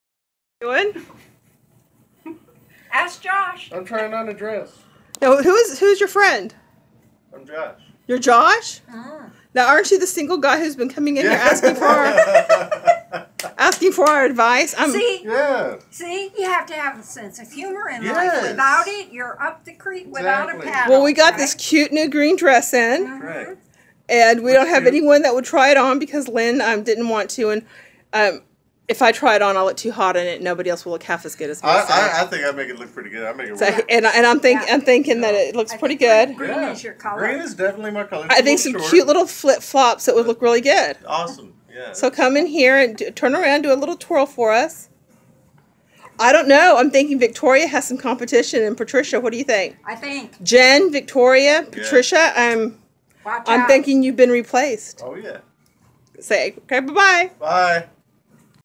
Ten of Arts in Downtown Carrollton Texas
Josh Dresses in a green dress.